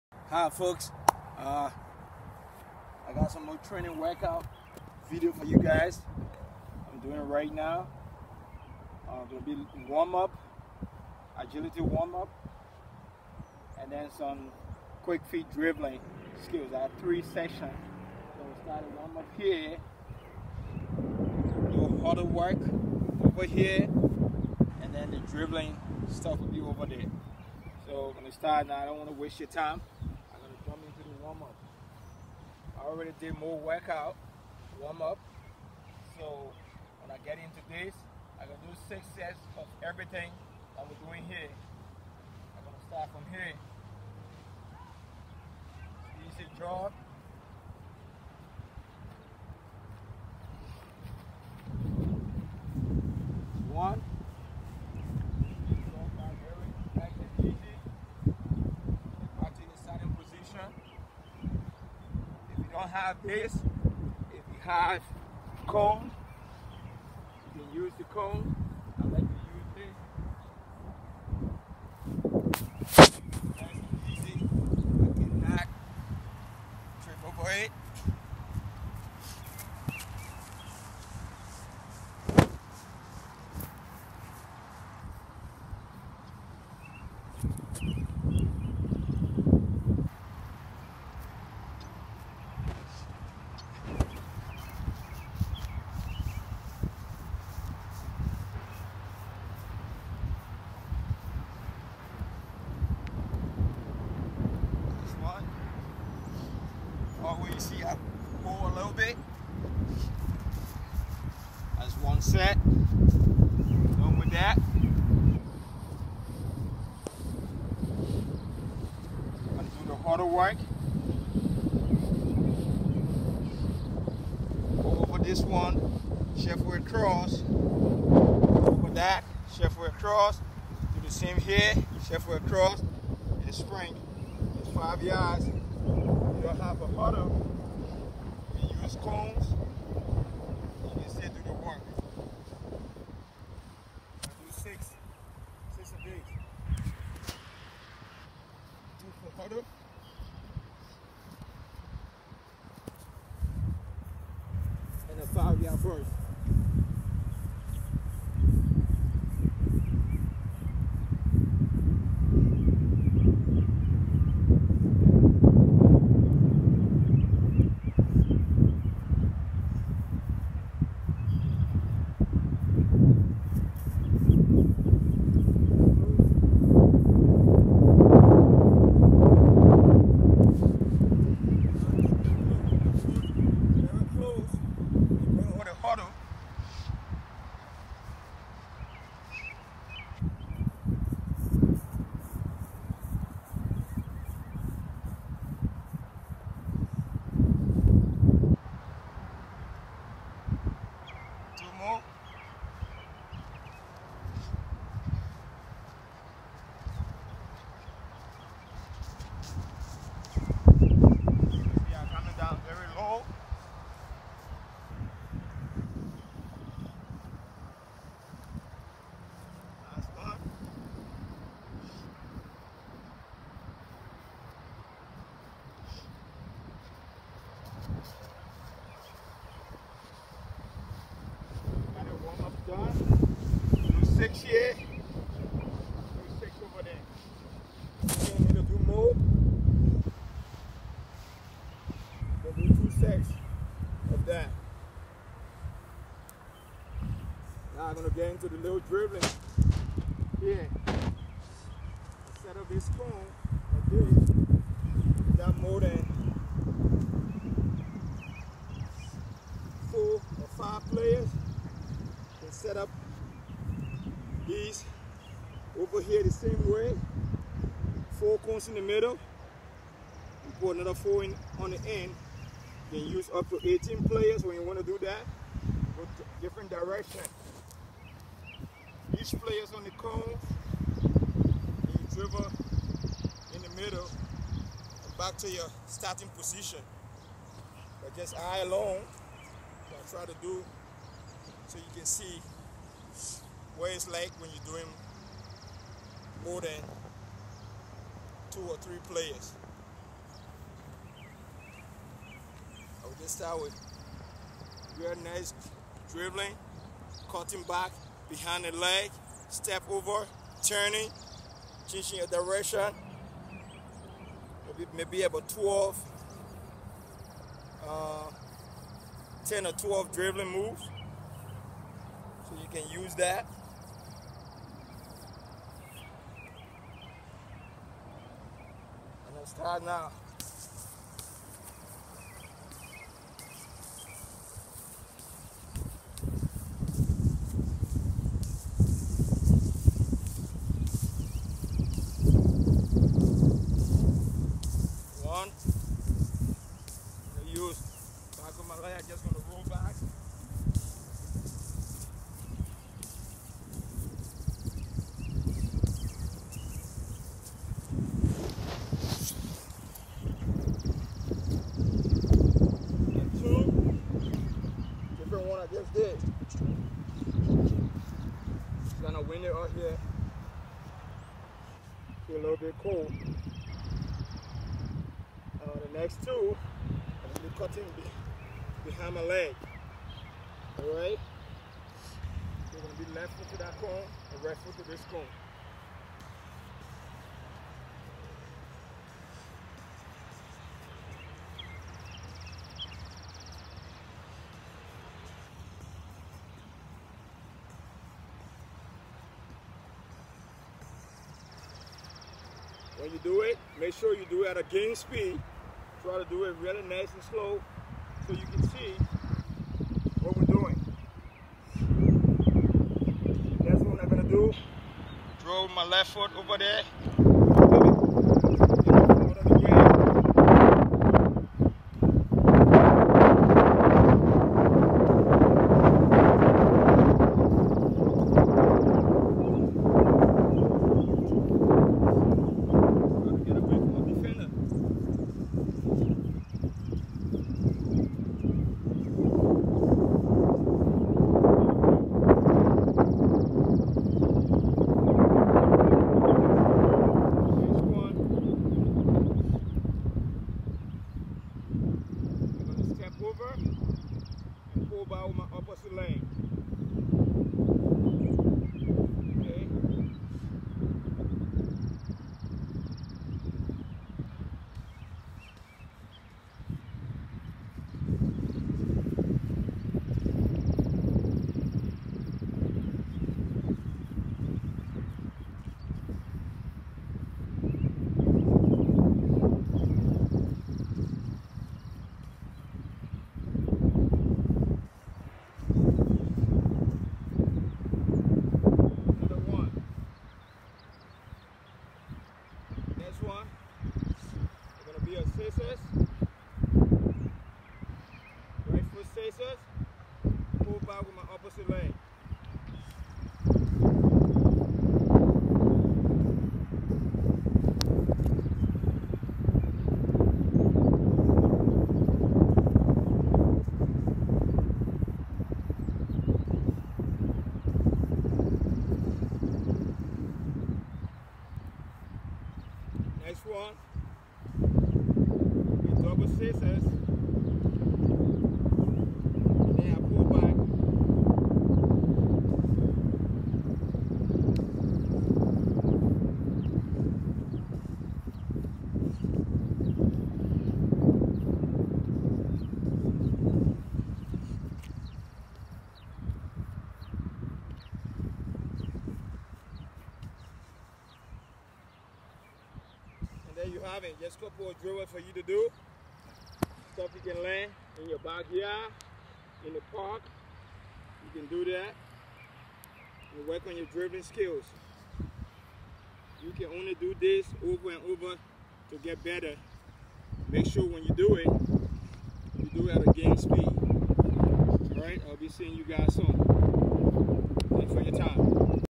Johnny Menyongar-Agility/dribbling
10 minute agility and ball dribbling workout hope you enjoy.